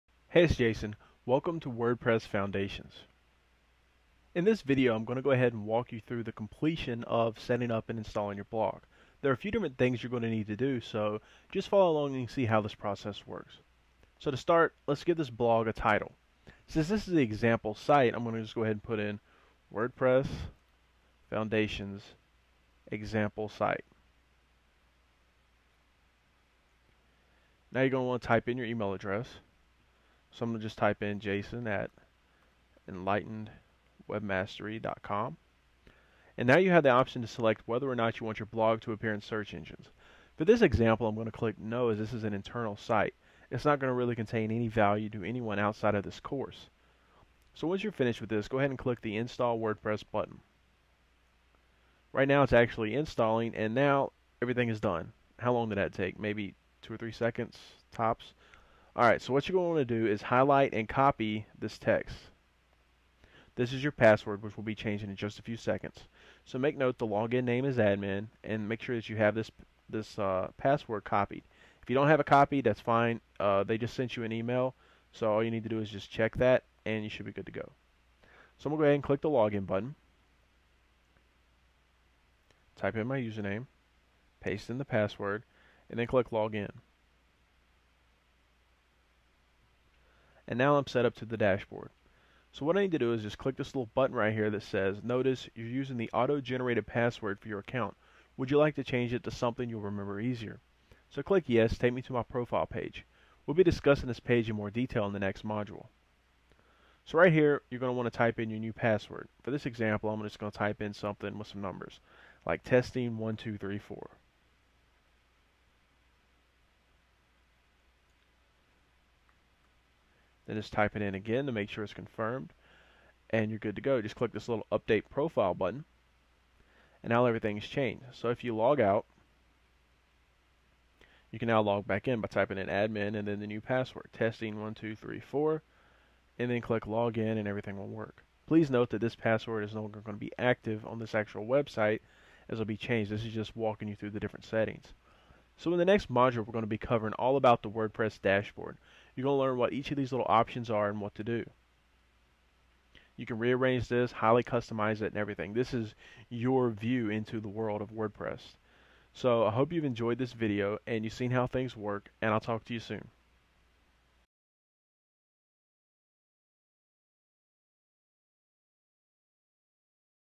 Live Blog Install
your A-Z course teaching you how to use WordPress

Here I install WordPress LIVE, see how quick and easy it is

If you want to learn more about the key blogging mindsets checkout